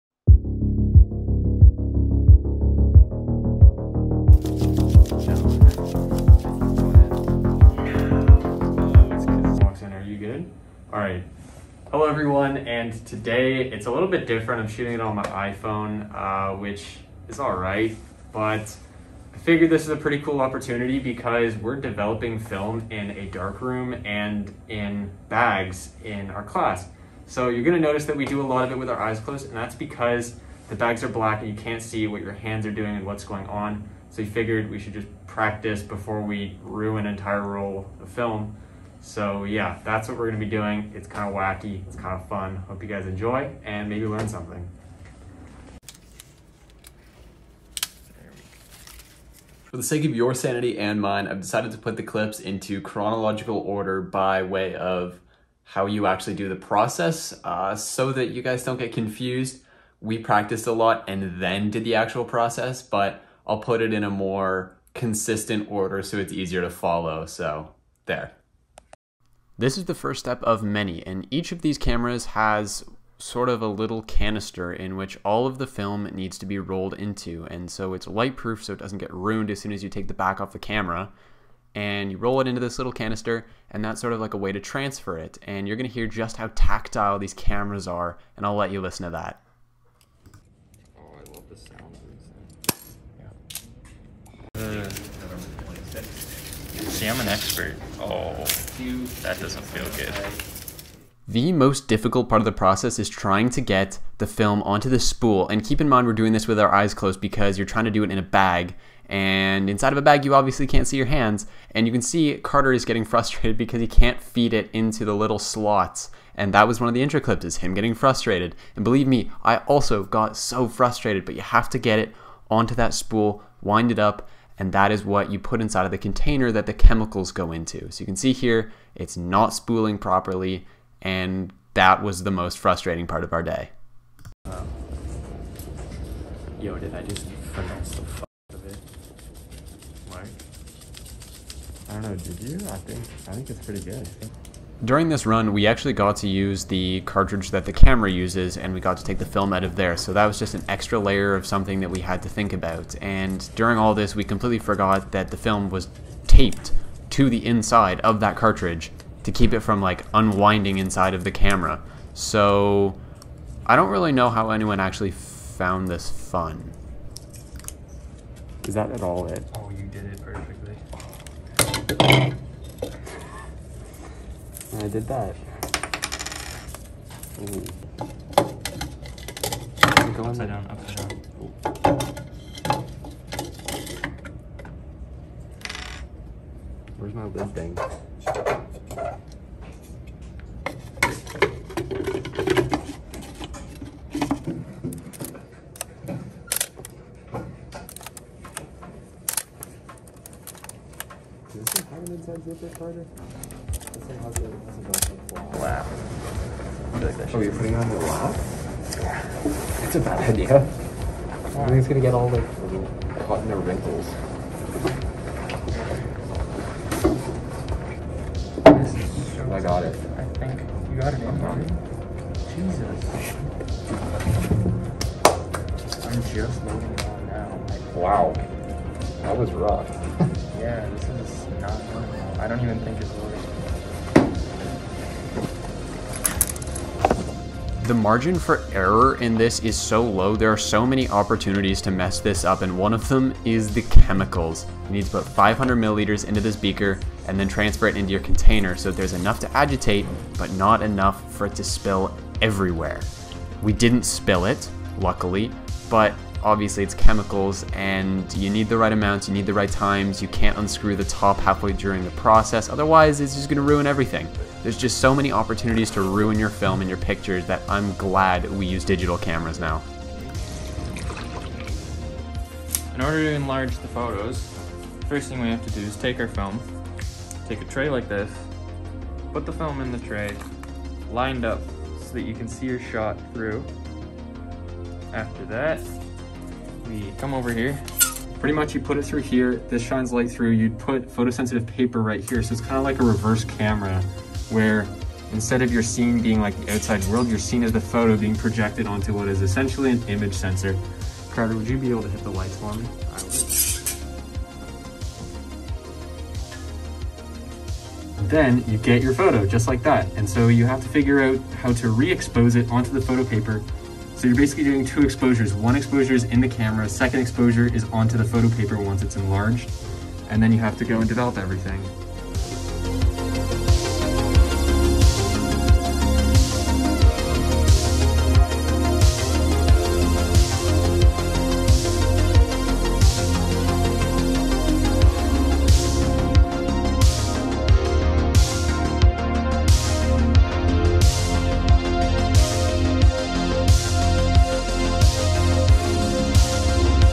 how to develop 35mm film (Told by 2 noobs)
WE SPENT ALL DAY ON THIS. Please leave us a like because honestly... we almost lost our collective tempers on this one. I know this video specifically isn't super well made but it was a cool opportunity. That and we were in class soooooo...